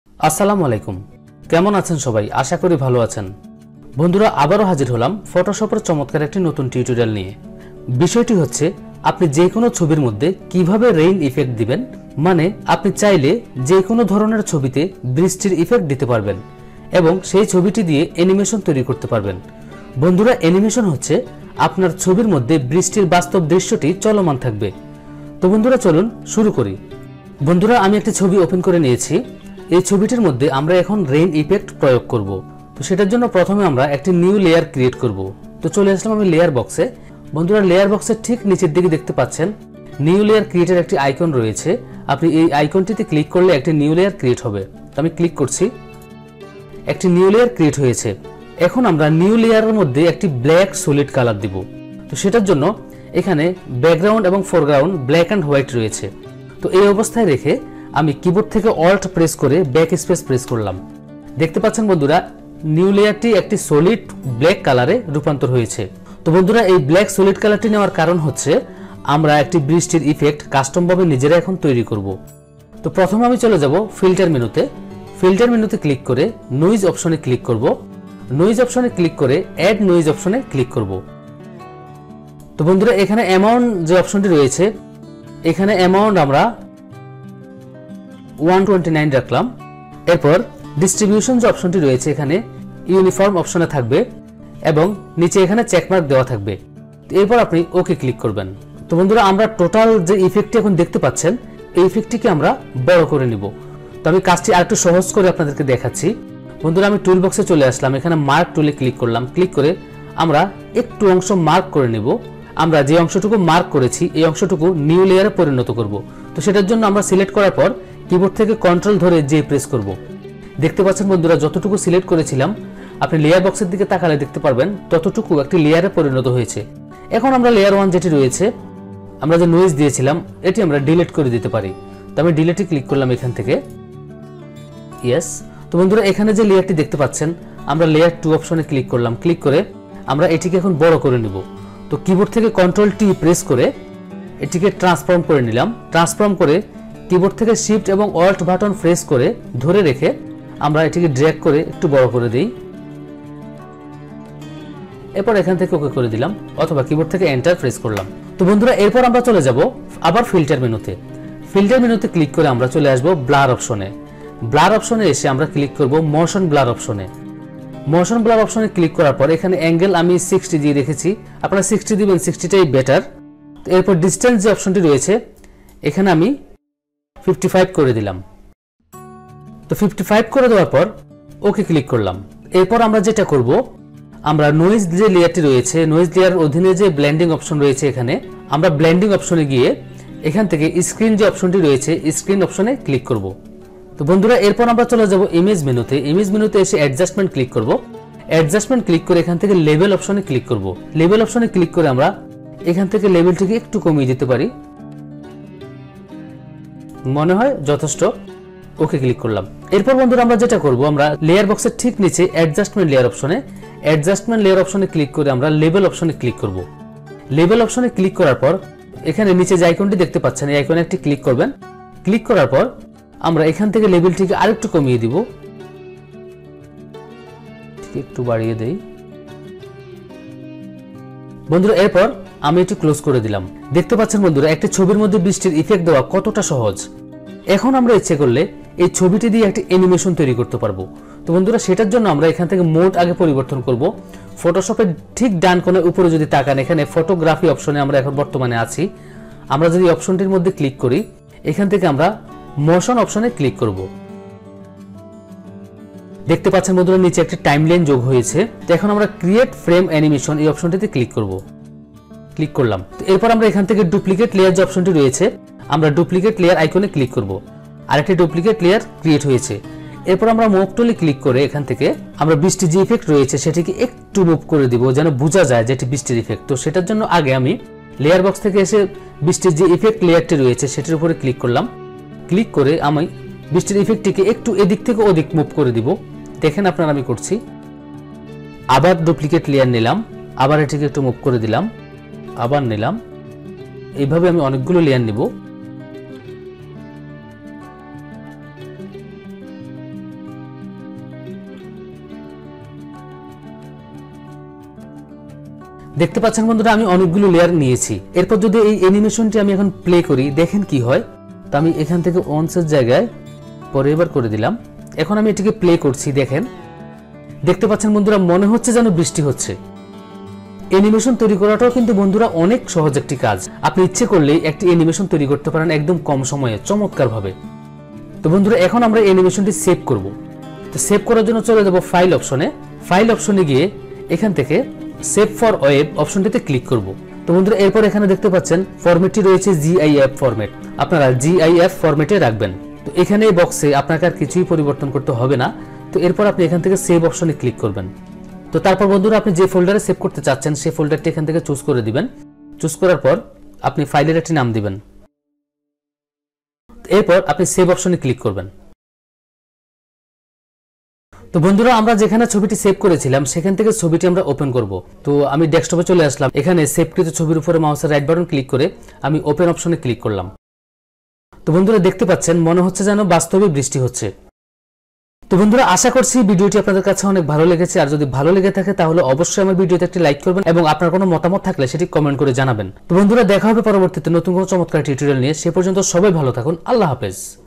How to Make Rain Effect Photo in Photoshop cc & Create Animation
In this tutorial, you're going to learn how to make a realistic Rain Effect in Adobe Photoshop CC 2014.